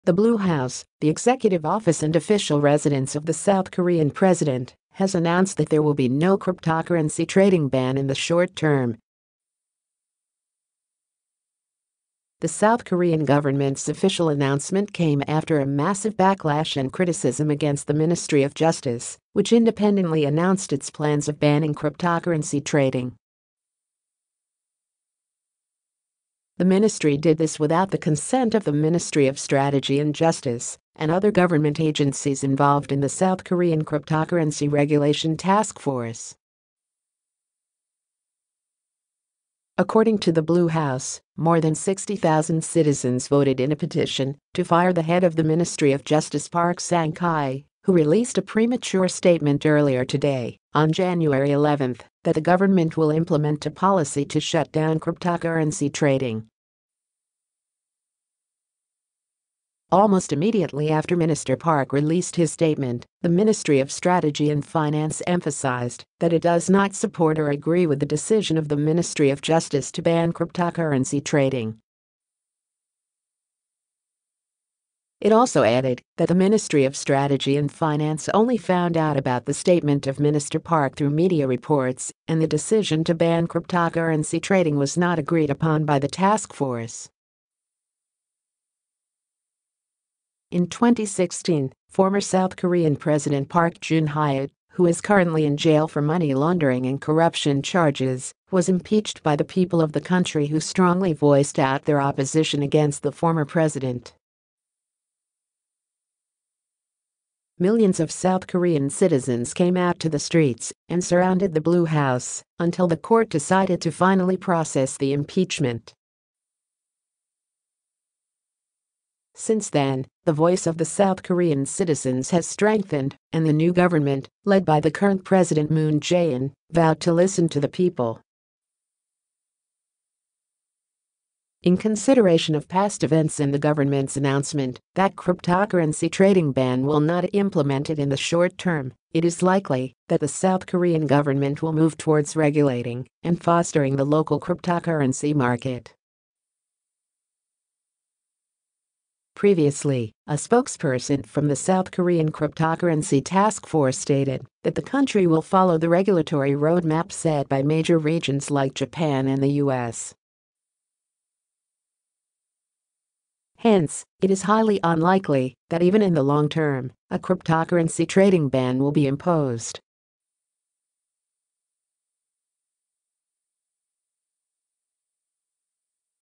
OFFICIAL: No Cryptocurrency Trading Ban in South Korea, Government Says
The Blue House, the executive office and official residence of the South Korean President, has announced that there will be no cryptocurrency trading ban in the short-term.The South Korean government’s official announcement came after a massive backlash and criticism against the Ministry of Justice, which independently announced its plans of banning cryptocurrency trading.The Ministry did this without the consent of the Ministry of Strategy and Justice and other government agencies involved in the South Korean cryptocurrency regulation task force.According to the Blue House, more than 60,000 citizens voted in a petition to fire the head of the Ministry of Justice Park Sang-ki, who released a premature statement earlier today, on January 11, that the government will implement a policy to shut down cryptocurrency trading.Almost immediately after Minister Park released his statement, the Ministry of Strategy and Finance emphasized that it does not support or agree with the decision of the Ministry of Justice to ban cryptocurrency trading.It also added that the Ministry of Strategy and Finance only found out about the statement of Minister Park through media reports, and the decision to ban cryptocurrency trading was not agreed upon by the task force.In 2016, former South Korean President Park Geun-hye, who is currently in jail for money laundering and corruption charges, was impeached by the people of the country who strongly voiced out their opposition against the former president.Millions of South Korean citizens came out to the streets and surrounded the Blue House, until the court decided to finally process the impeachment.Since then, the voice of the South Korean citizens has strengthened and the new government, led by the current President Moon Jae-in, vowed to listen to the people.In consideration of past events and the government’s announcement that cryptocurrency trading ban will not implemented in the short-term, it is likely that the South Korean government will move towards regulating and fostering the local cryptocurrency market.Previously, a spokesperson from the South Korean cryptocurrency task force stated that the country will follow the regulatory roadmap set by major regions like Japan and the US.Hence, it is highly unlikely that even in the long-term, a cryptocurrency trading ban will be imposed.The spokesperson said:.